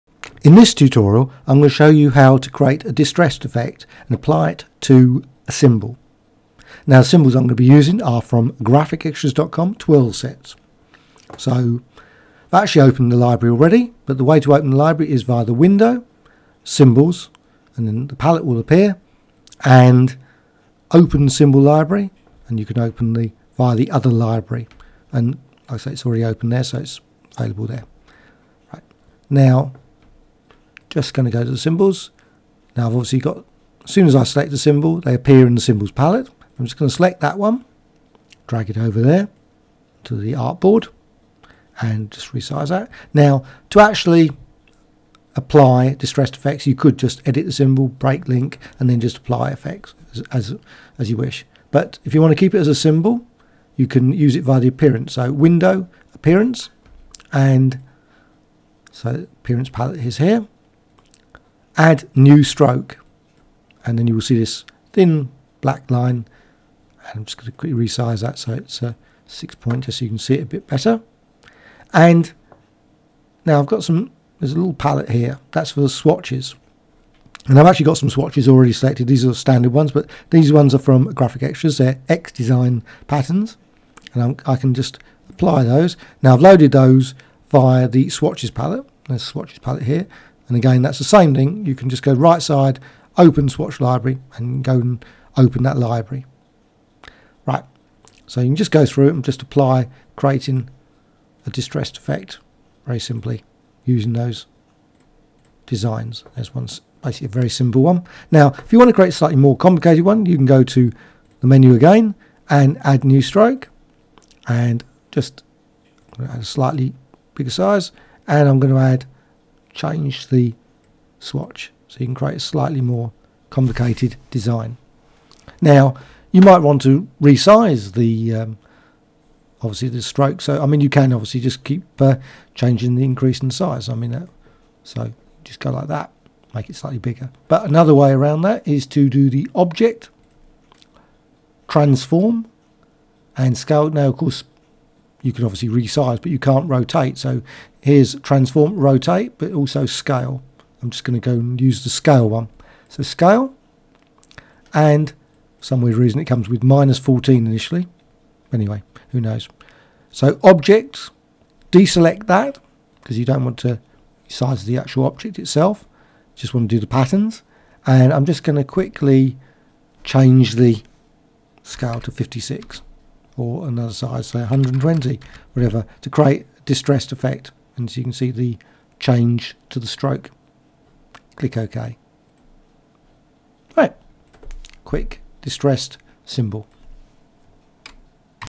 How To Apply A Vector Distress Roughen Effect In Illustrator
Tutorial to show a distress effect applied to a symbol (without changing it to a path) in Illustrator  (though same approach can be used in CC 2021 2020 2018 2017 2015  CS6 CS4 etc PC or mac) The tutorial shows the use of the appearance palette and others.